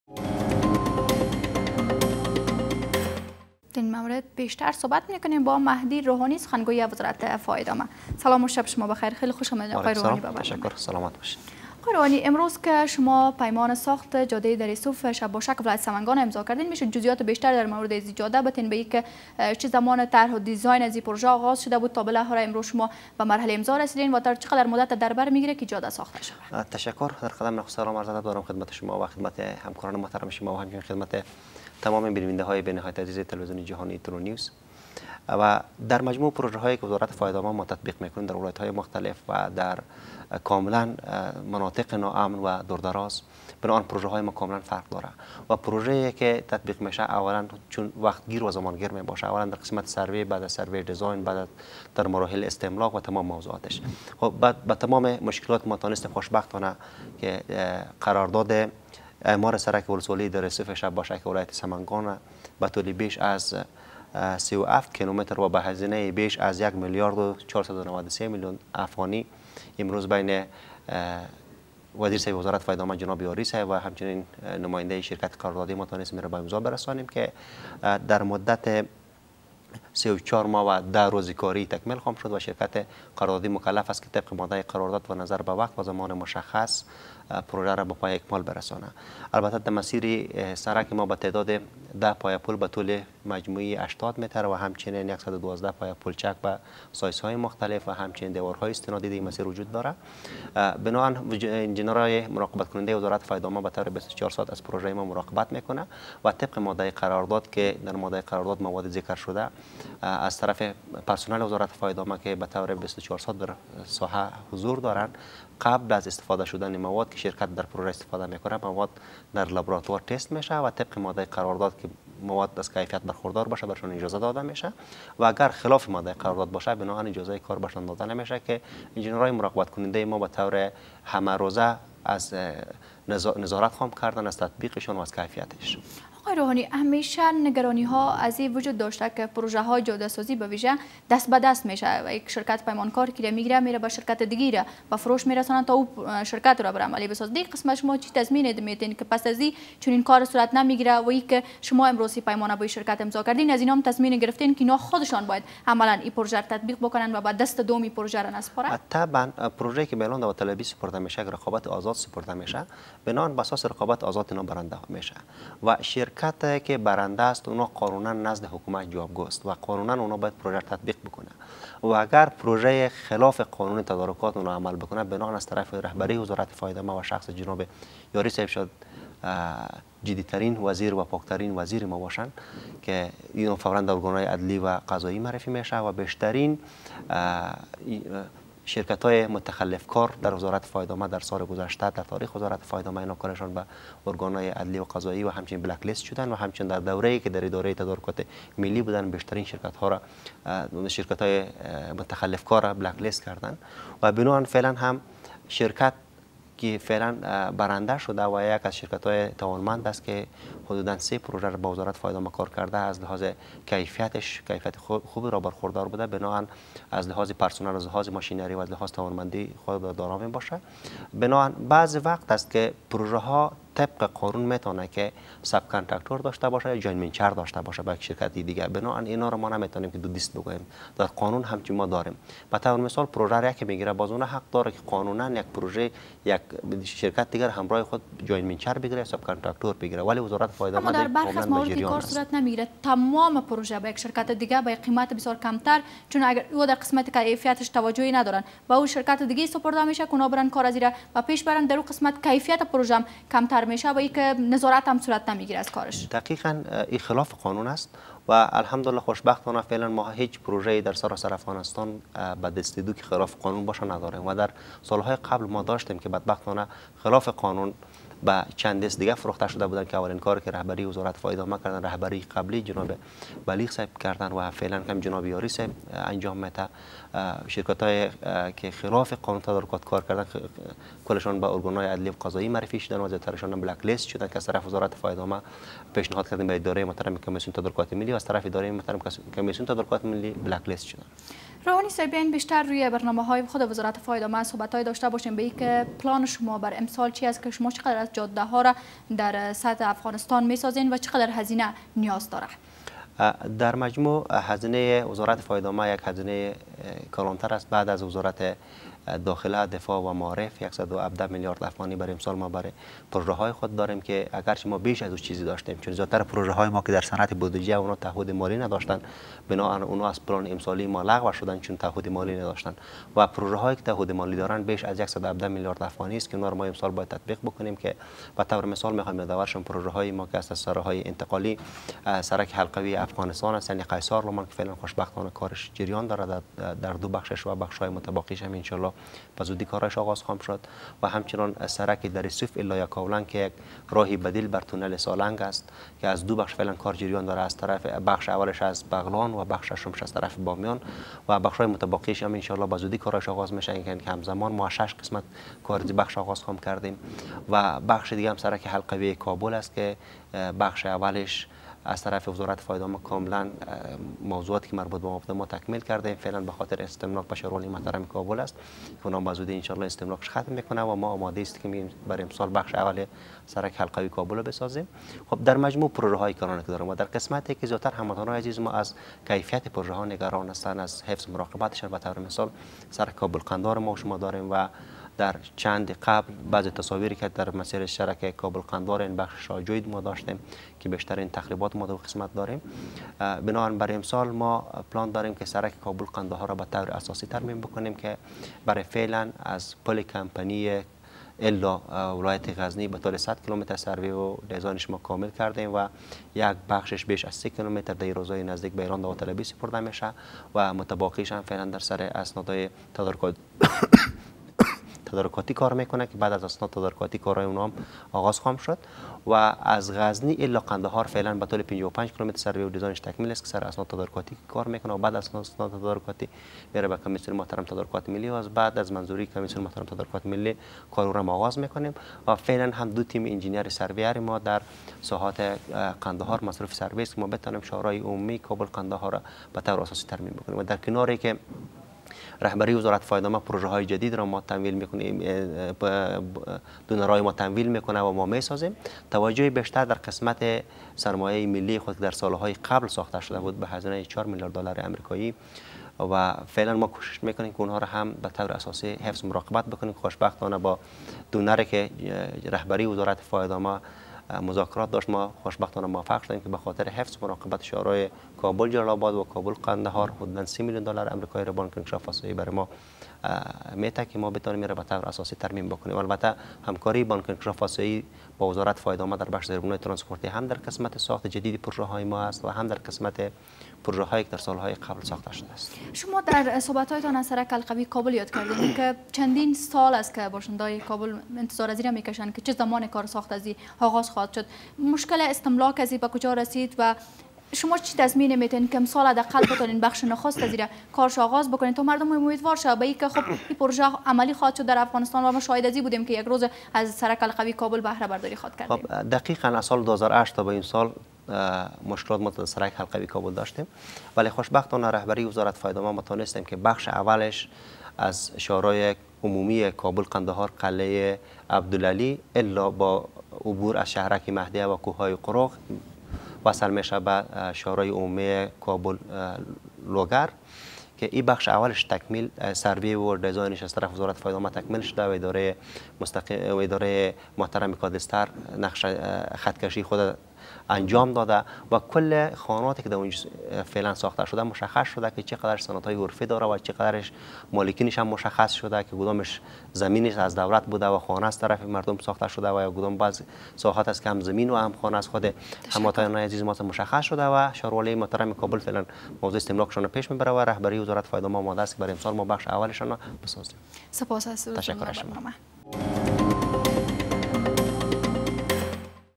بازار: امضاء پیمان ساخت جادۀ دره صوف سمنگان
وزارت فواید عامه پیمان ساخت جاده دره صوف شب باشک ولایت سمنگان با درازای سی و هفت کیلومتر با یک شرکت خصوصی امضاء کرد .